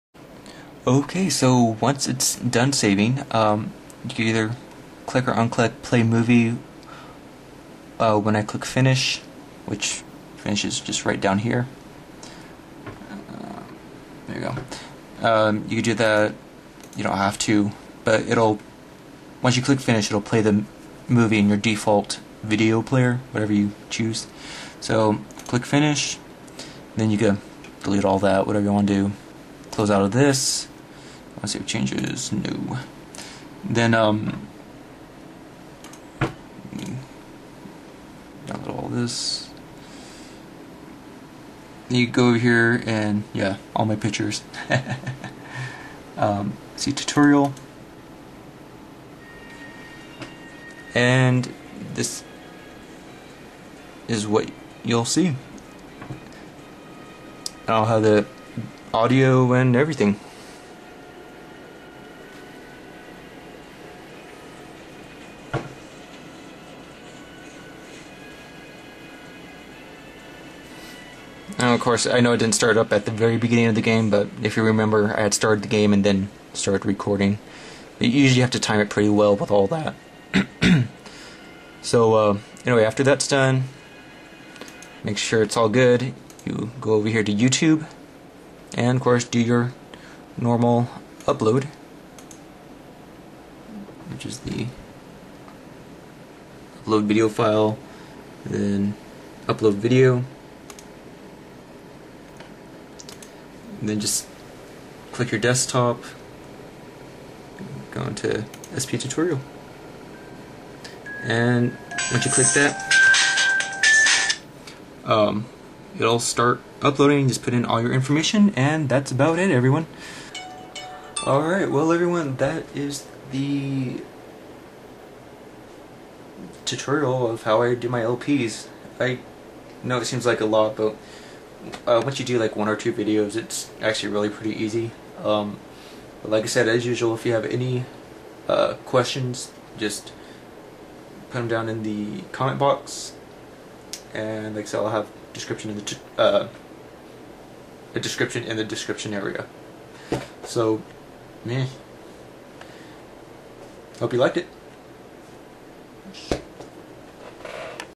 SilentPhill's LP Tutorial PT3!
Finishing up...

STEP 26: After it has saved, click FINISH.

STEP 27: Go onto YouTube.com, and upload your video!!!

DONE!

As stated, if you have ANY questions about this method, then just post them in the comments section!